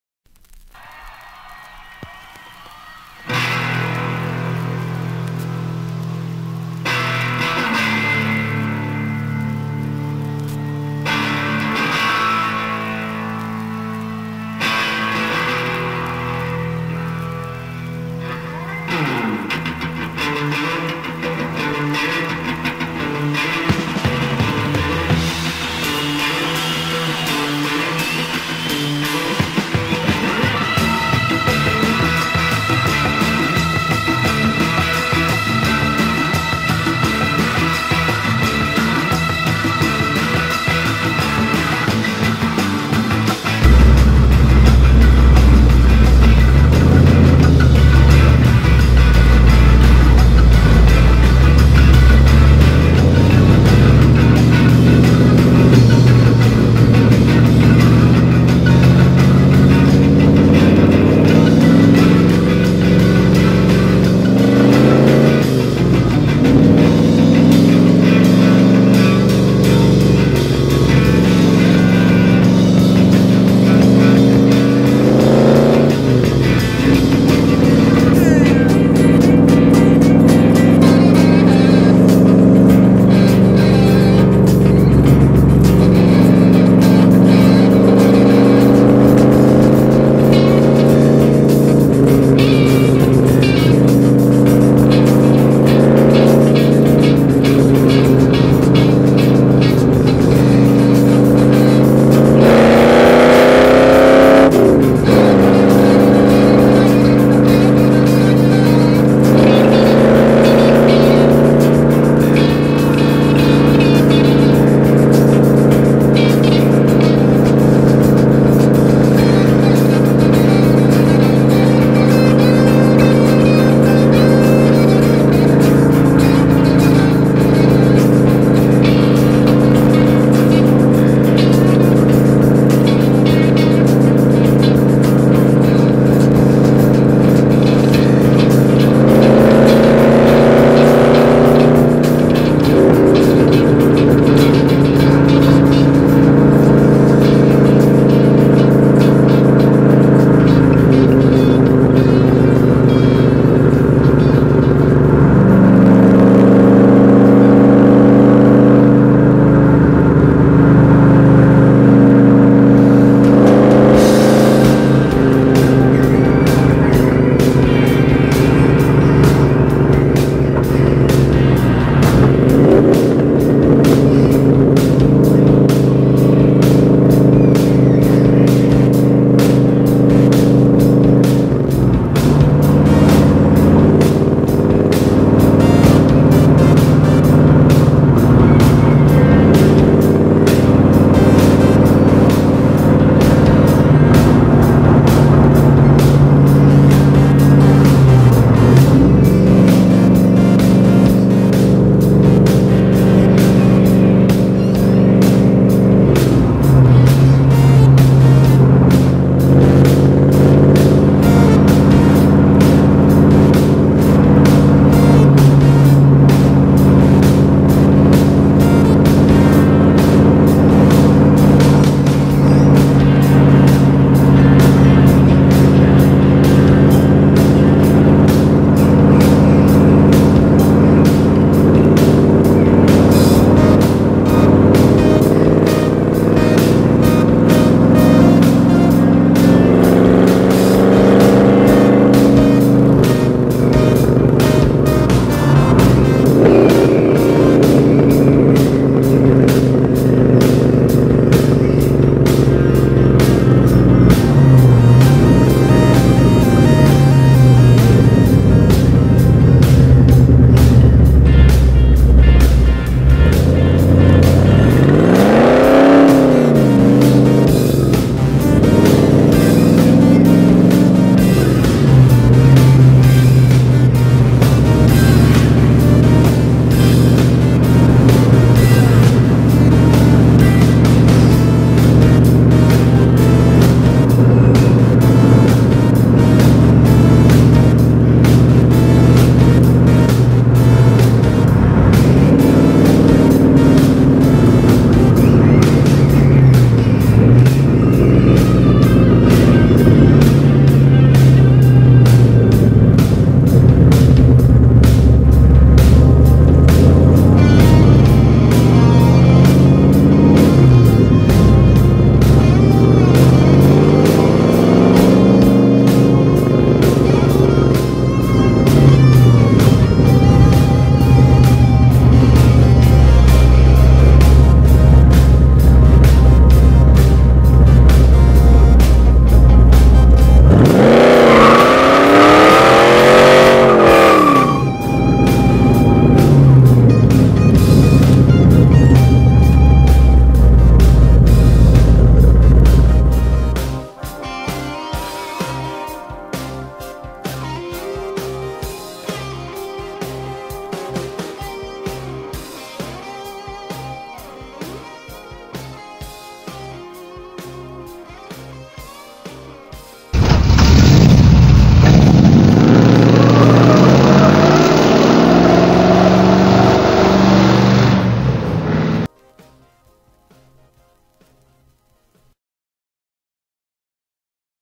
Another ride on a 1971 Dodge Challenger
Another few minutes riding around Vancouver Washington on the side of a 1971 Dodge Challenger; freeway run and a nice smoke show at the end.  Episode VI, Revenge of the Challenger.  Episode IV, A new hope - dealing with the death of two Mopars and the rebuilding of this '71 Challenger - coming soon (due in February, 2009).